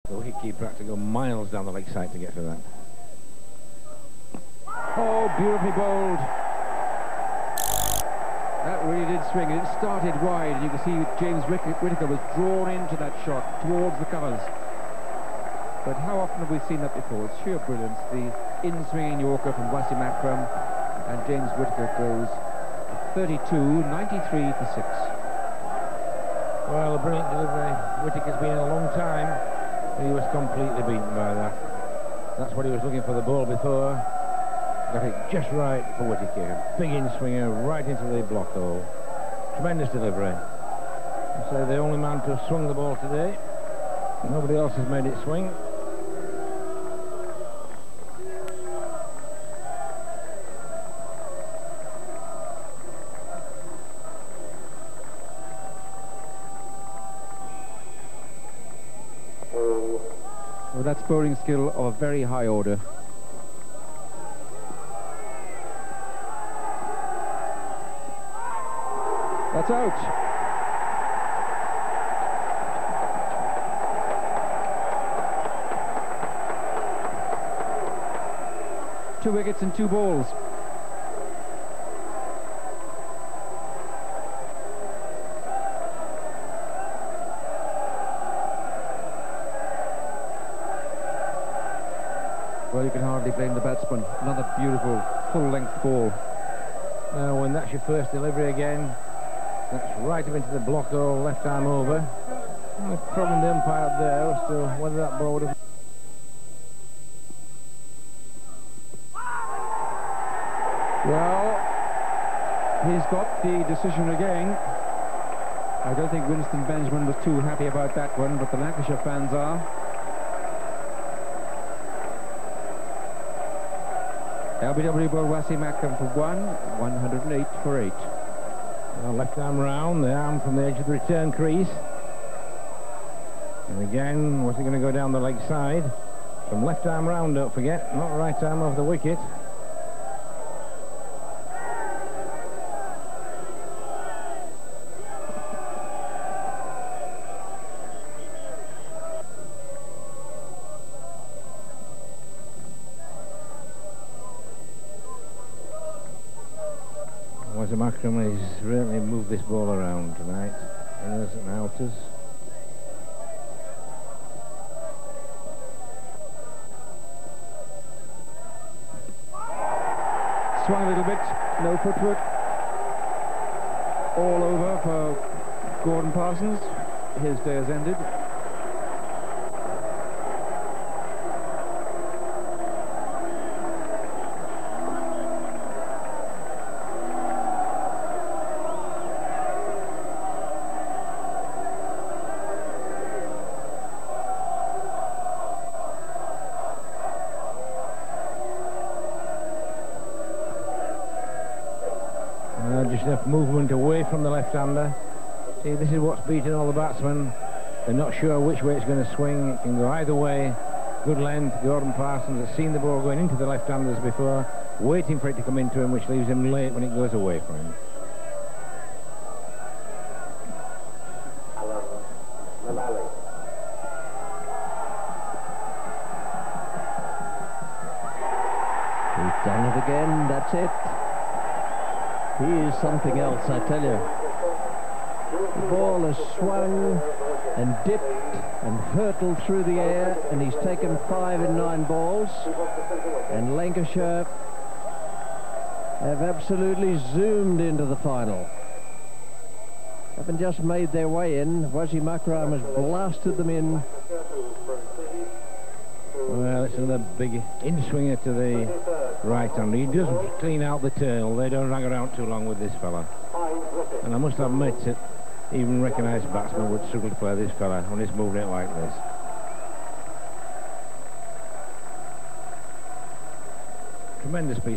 *RARE* Wasim Akram 5-10 of 7 Overs vs Leicestershire County Match 1993
The art of swing bowling is at its best.
This was a Semifinal between Lancashire and Leicestershire played at leicester ground  of Benson & Hedges cup 1993.